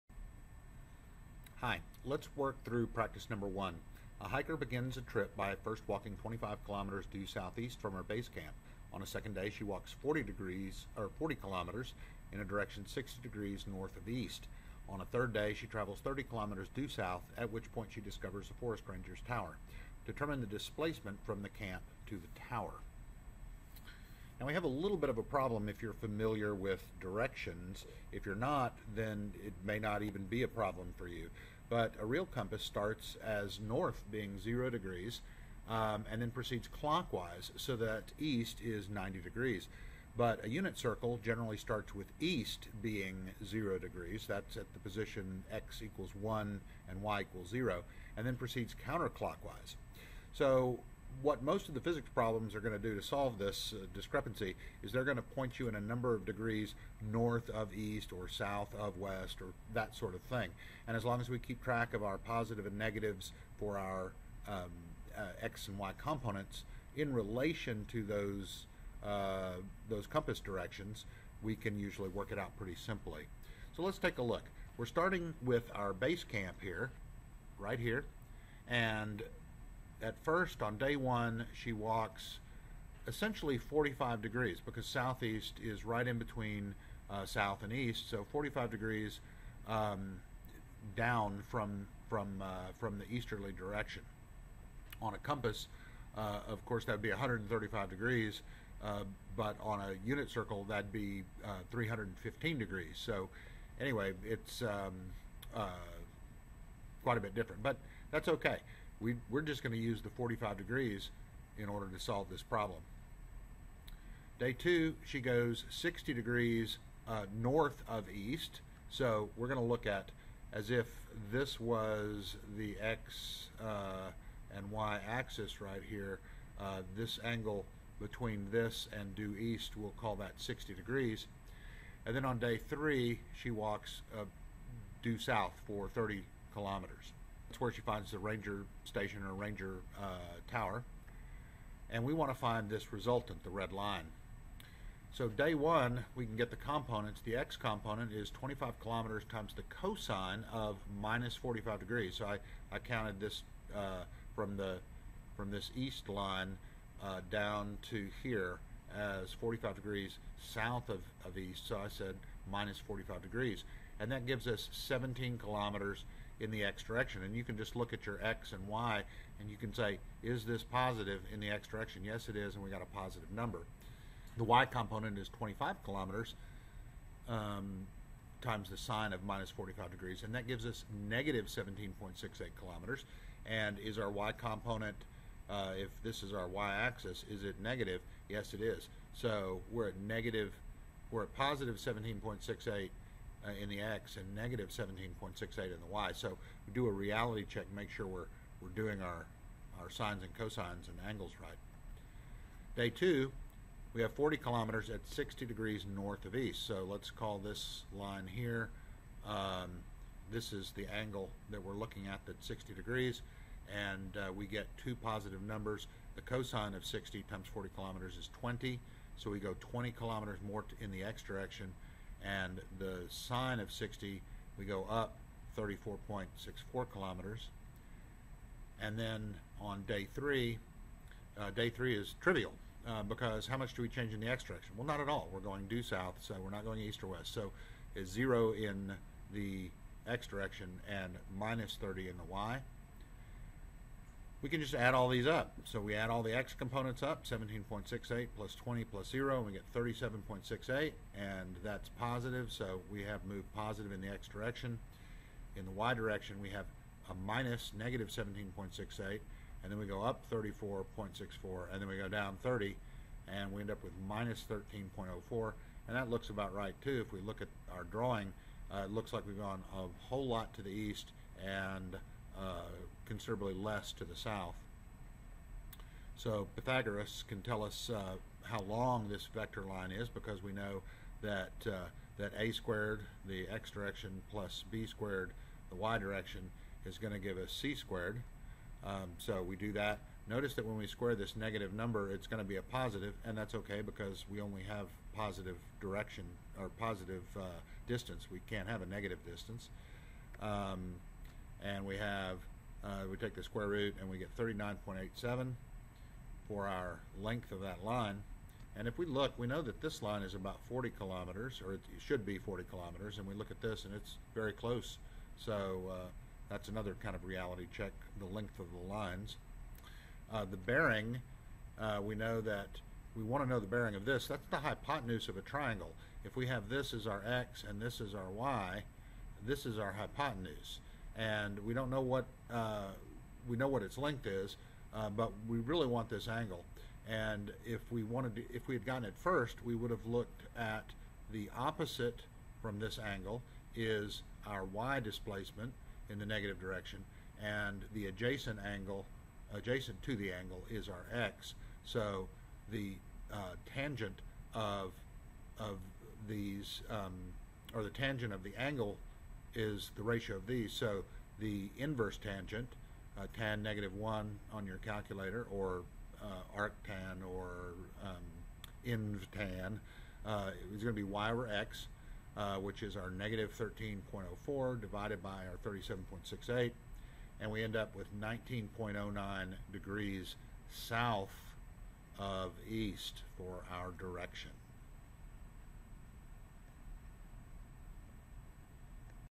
Physics: Hiker Vector Problem Worked
A hiker walks three directions on three different days.  Find the resulting displacement.  This is an example problem worked out, so that you can check your work.